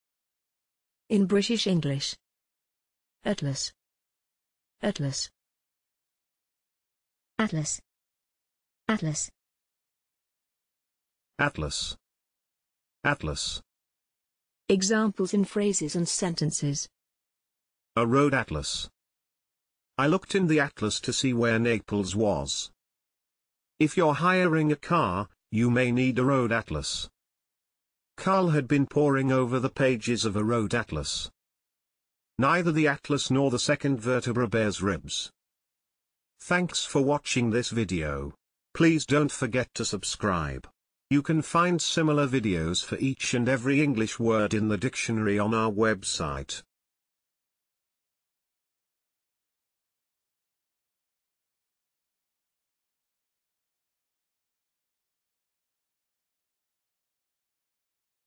atlas - pronunciation + Examples in sentences and phrases
This is the pronunciation of atlas in British English + a few examples of its pronunciation in phrases and sentences.